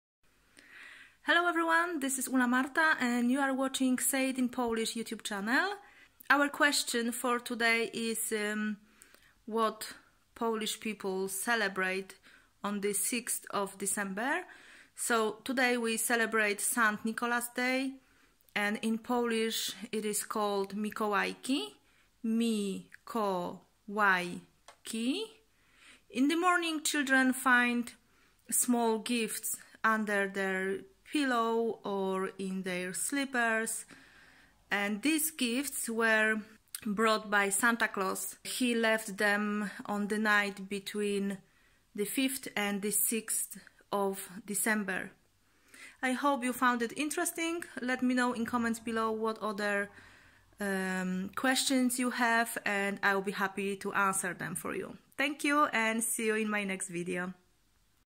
How Poles celebrate 6th December - Mikolajki - SAY IT IN POLISH
This a  short video expaining how Polish people celebrate  St. Nicolas Day in Poland.